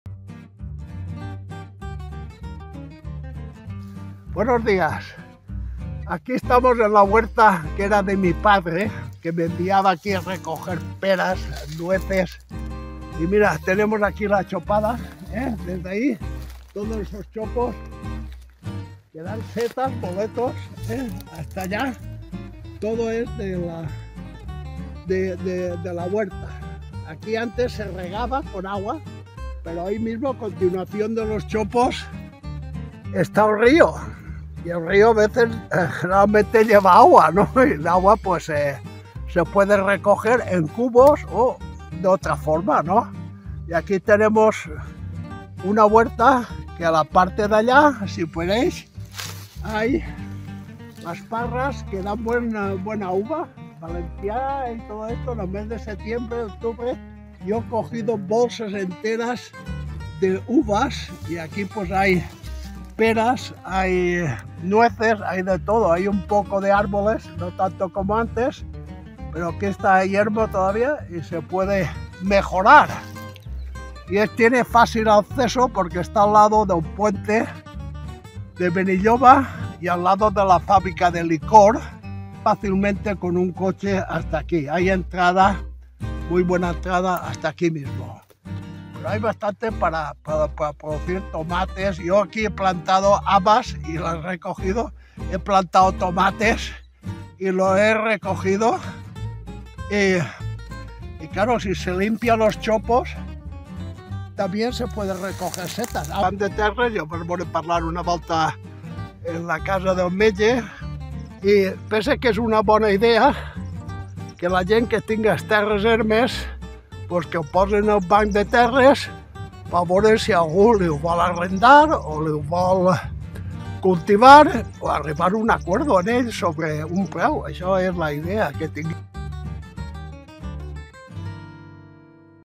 HORTA PARTIDA DE LA CREU BDT BENILLOBA
BANCO DE TIERRAS DE BENILLOBA. PARCELA HORTA PARTIDA DE LA CREU 1.301 m2 . Propietario Fernando Espinós